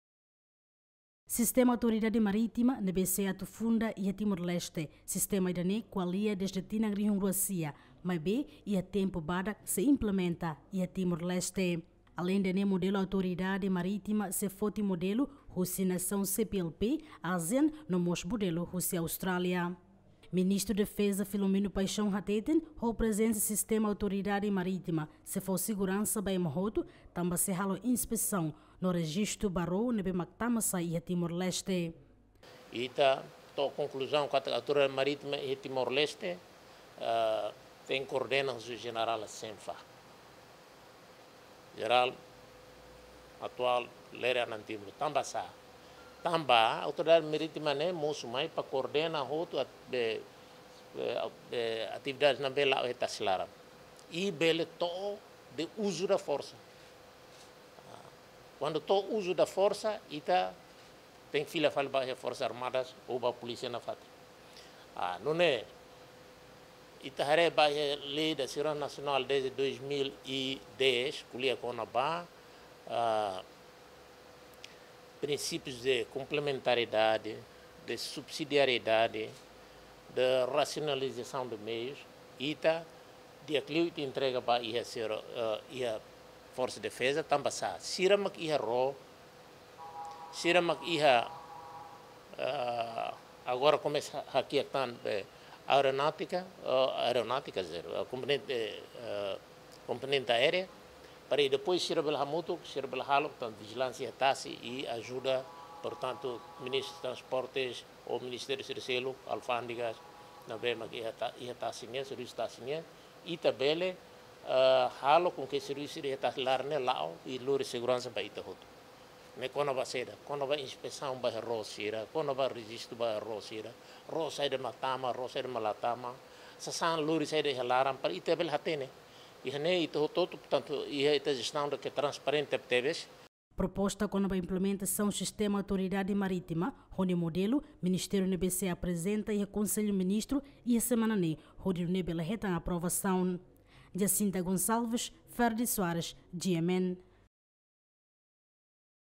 Governu sei Estabelese sistema Autoridade Marítima ba Atividade Tasi Laran
Atu halo Vijilánsia ba Atividade Tasi Laran Governu sei Estabelese sistema Autoridade Marítima